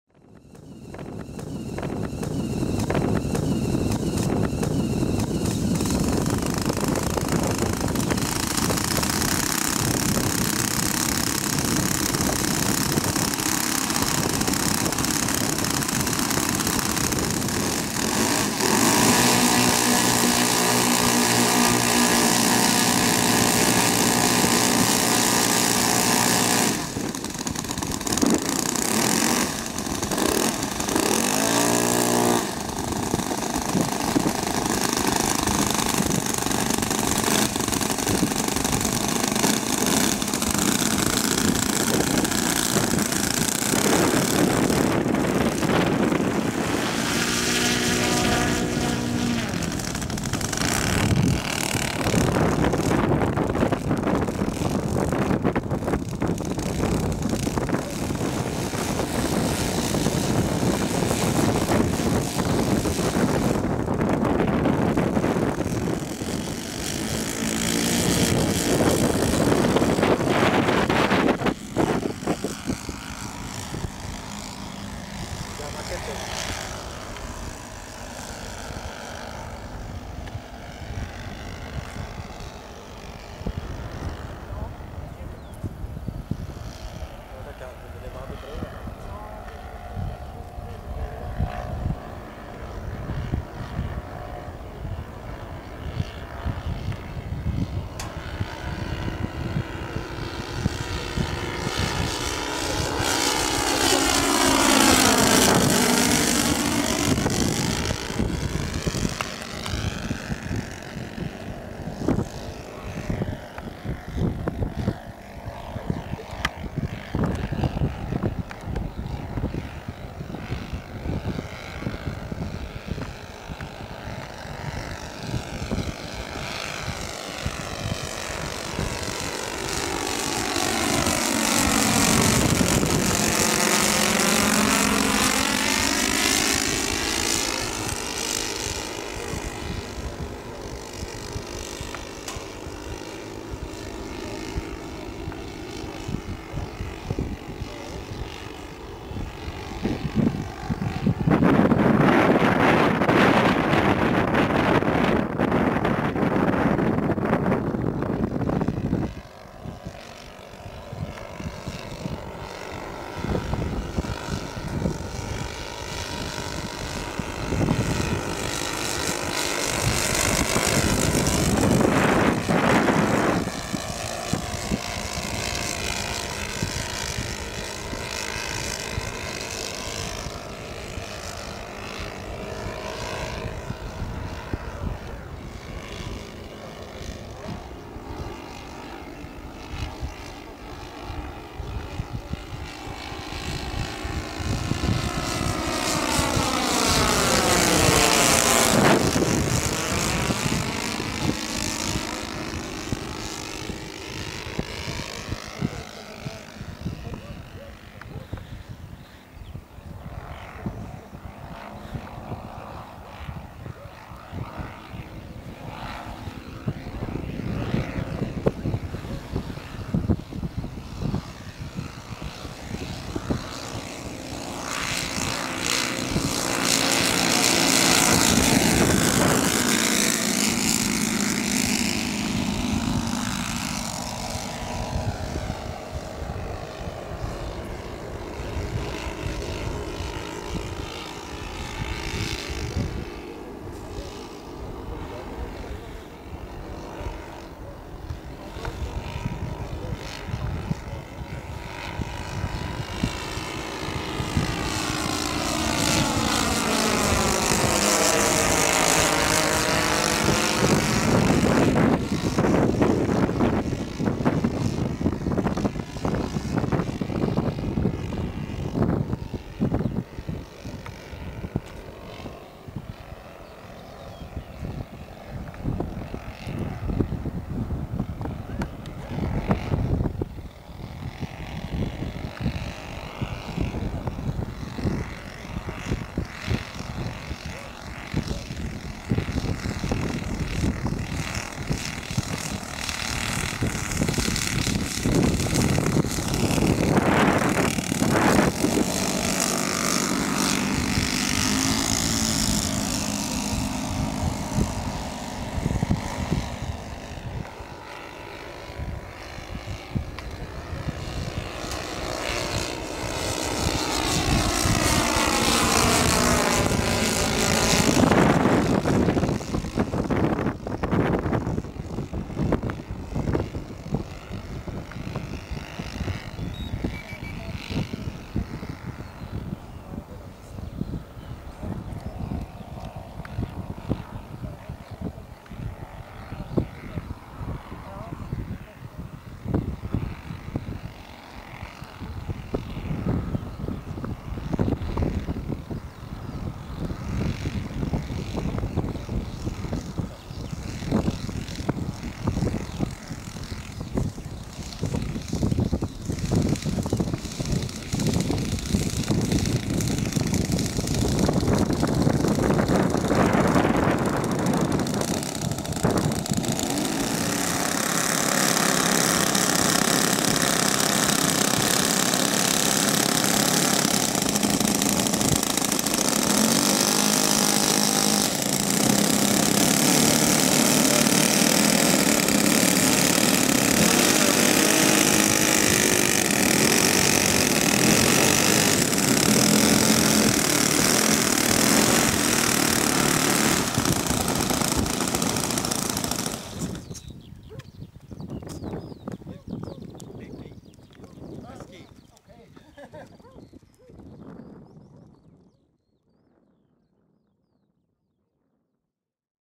RC Junkers Ju-52, 1:7.8 Scale
Junkers Ju-52, 1:7.8 Scale, Wingspan 3,75m, three 4-stroke engines displaced at 20 ccm each, fiberglass construction - flying at 10kts wind, May 5th, 2012